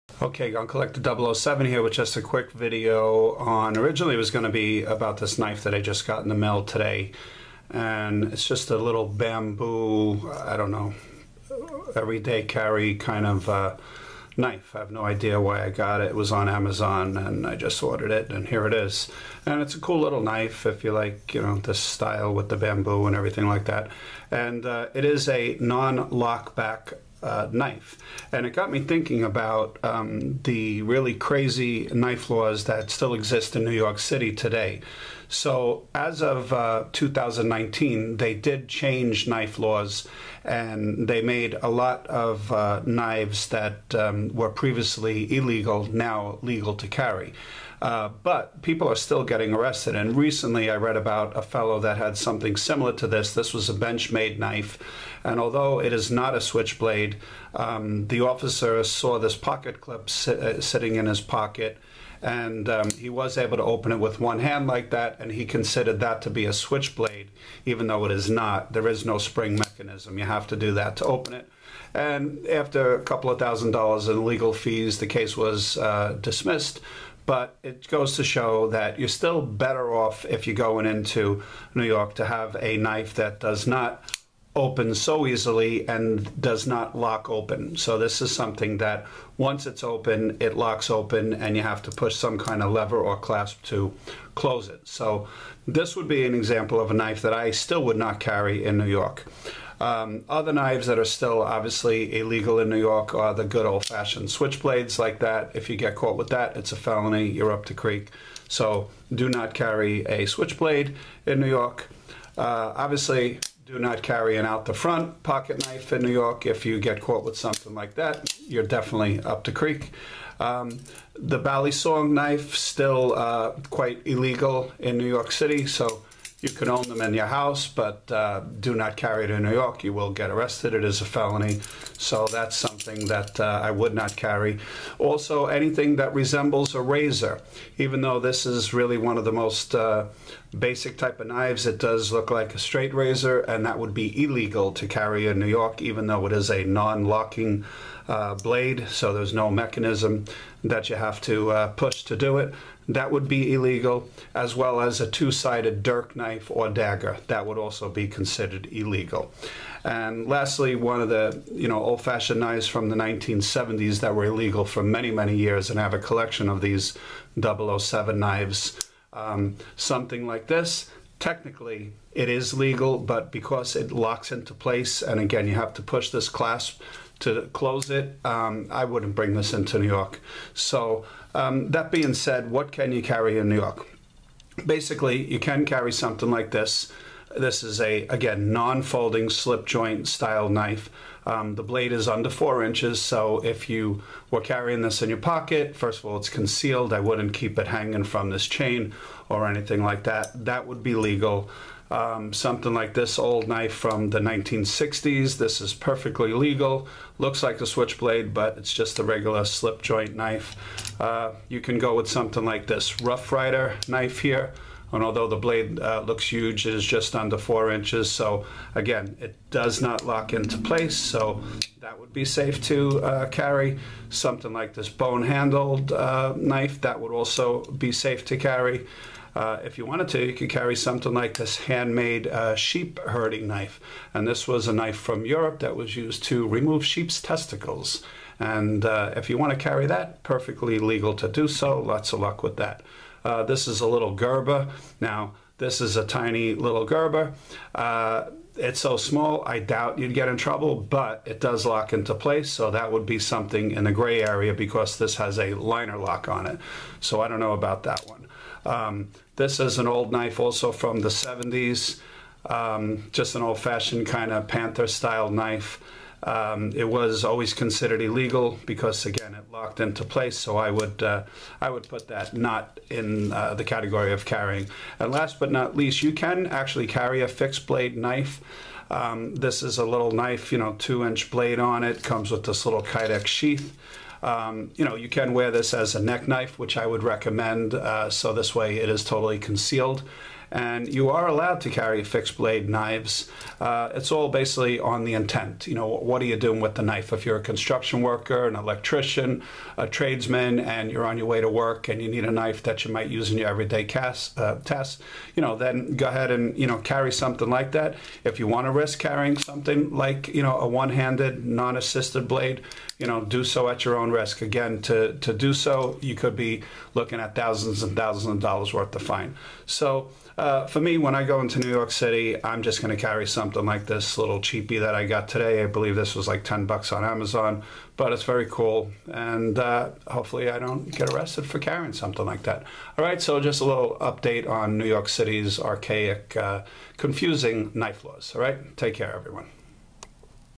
New York City Legal Pocket Knife Update!
Update on New York City pocket knives that are legal and illegal

Pocket knives that are Legal to carry and knives that are Illegal to carry in NYC

link to razor folding knife

Nyc pocket knives

Bamboo folding pocket knife